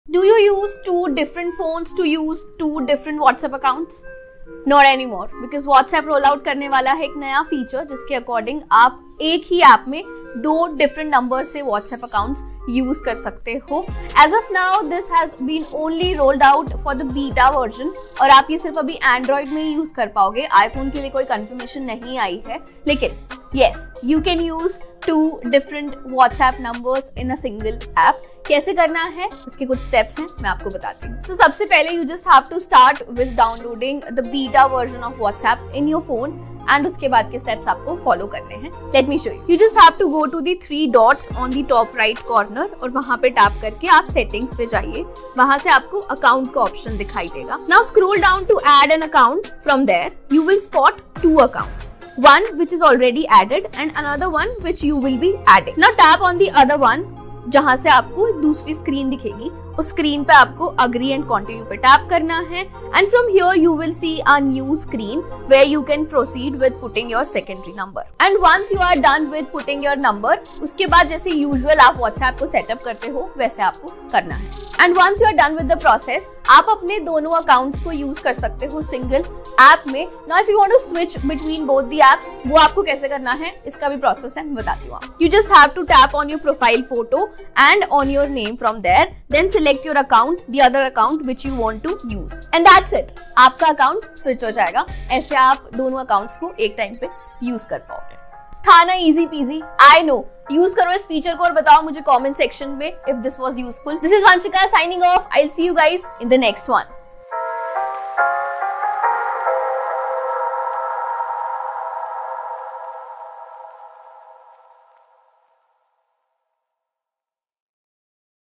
WhatsApp's Dual Number Feature Hits Android Soon | Here's how you can access it | Gadget Times
The feature will be available for Android users in the coming weeks. Once available, users will be able to use and switch two mobile phone number WhatsApp accounts in one app.